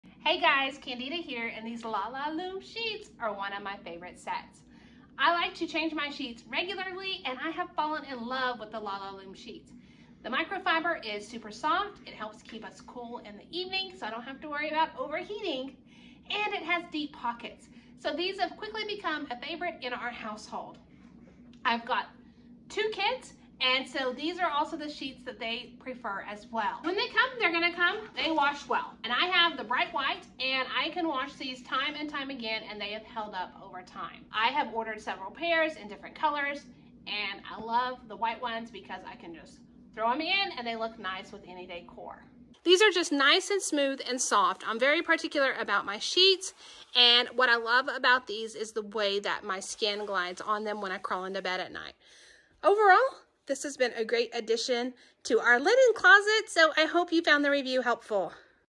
lalaLOOM Queen Bed Sheet Set Review | Soft Microfiber Luxury Bedding | Deep Pocket | 4 Piece Set
Check Here For Best Price

Welcome to our YouTube video review of the lalaLOOM Queen Bed Sheet Set. Experience the ultimate in comfort and luxury with this soft microfiber bedding. With an extra deep pocket design, this 4-piece sheet set ensures a perfect fit for your queen-sized bed. The bright white color adds a touch of elegance to any bedroom decor. These sheets are breathable, wrinkle-resistant, and fade-resistant, ensuring long-lasting quality. In this video, we'll showcase the lalaLOOM Queen Bed Sheet Set, discussing its softness, durability, and overall performance.  Upgrade your bedding experience with lalaLOOM!